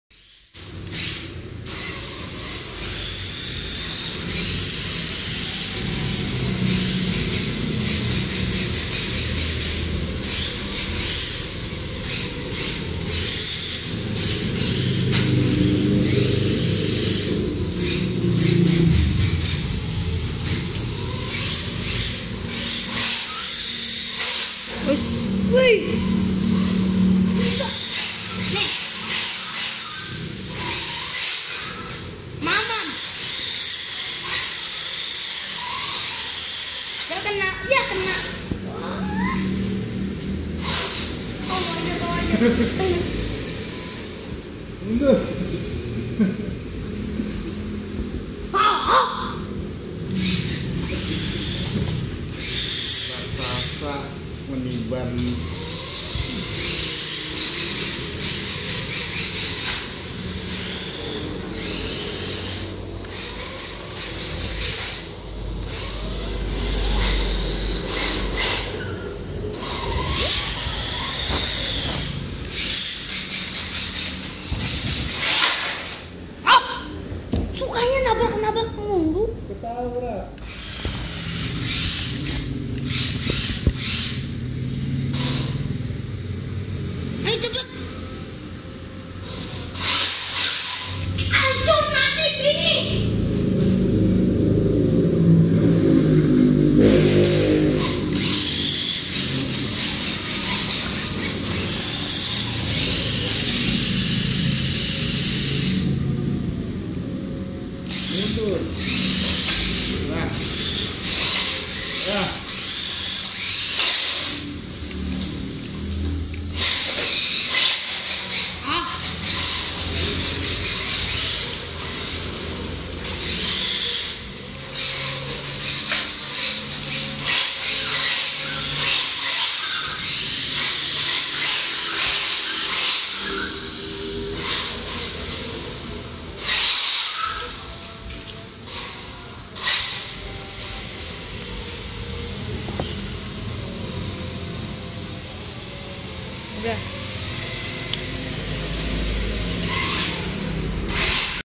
Kyosho Inferno MP9e Evo 1/8 vs UJIE The King Cheetah Turbo 1/16
Perbandingan Kyosho Inferno MP9e Evo 1/8 vs UJIE The King Cheetah Turbo 1/16